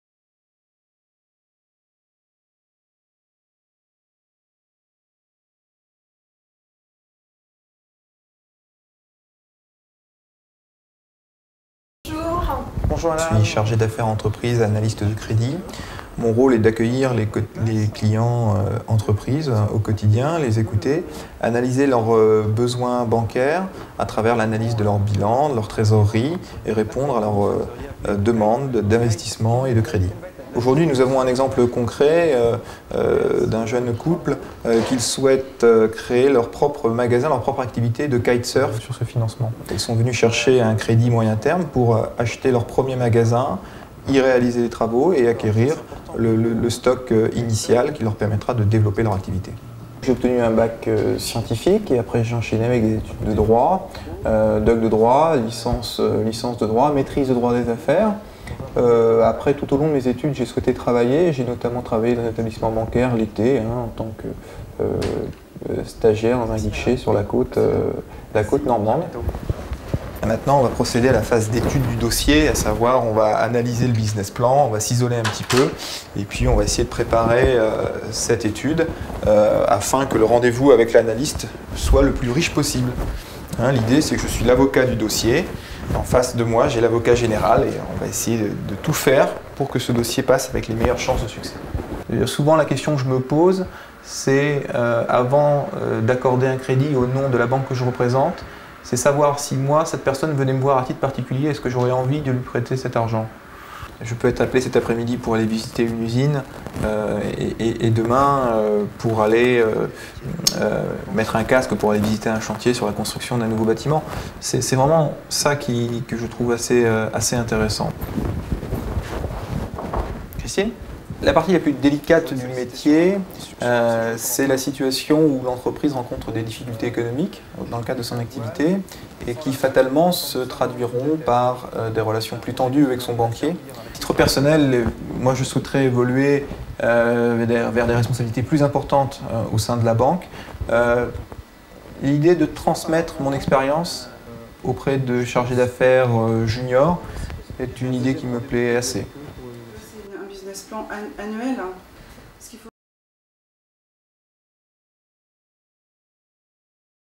Analyste de crédit
Tout dossier de demande de crédit passe entre les mains de l’analyste de crédit : c’est lui qui évalue le sérieux du projet et la solidité financière du client.